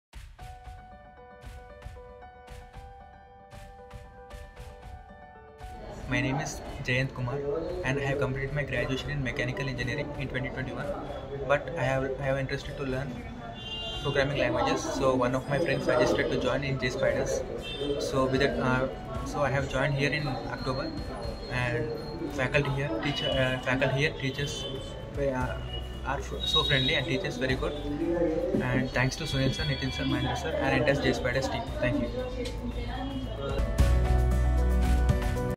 JSpiders Hyderabad JNTU | Student Testimonial | Got Placed As Java Developer
JSpiders Hyderabad Jntu
Congratulations to P  JAYANTH KUMAR
Got Placed in company​​​​​​​​​​​​​​​​ code CAP0122JSP02
Designation : Java Developer
HE is Sharing His Experience About
JSpiders Hyderabad JNTU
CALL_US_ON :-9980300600